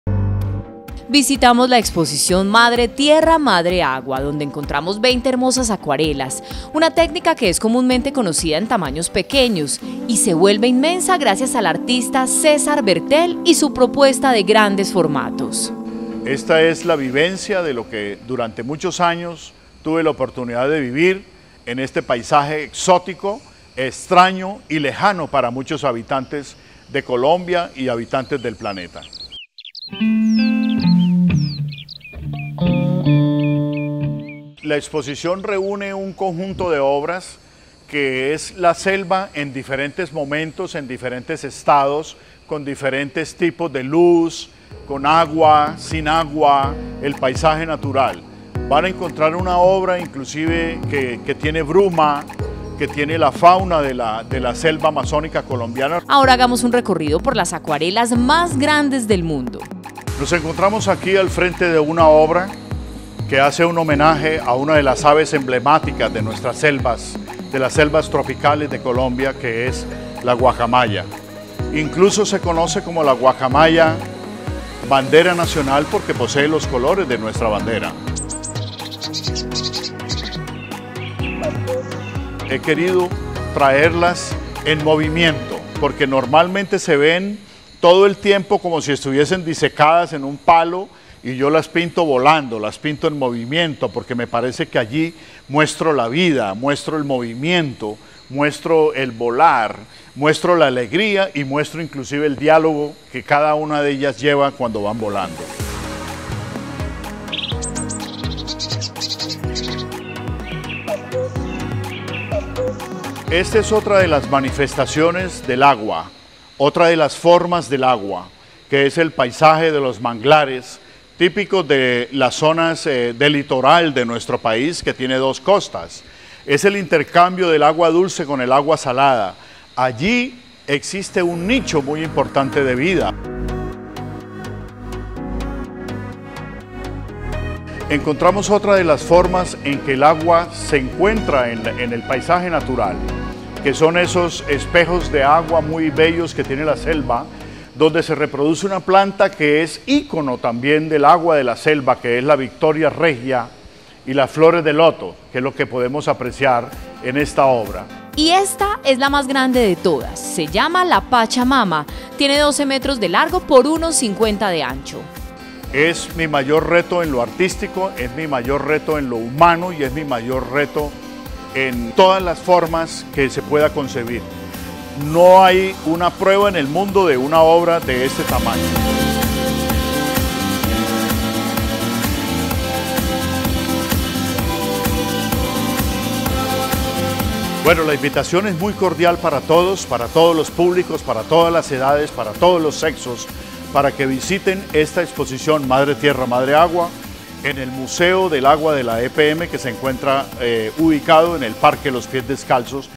Las acuarelas más grandes del mundo están en Medellín y son elaboradas por un colombiano
Visitamos la exposición Madre tierra, Madre agua, donde encontramos 20 hermosas acuarelas.
Una técnica que es comúnmente conocida en tamaños pequeños, se vuelve inmensa  gracias al artista César Bertel y su propuesta de grandes formatos.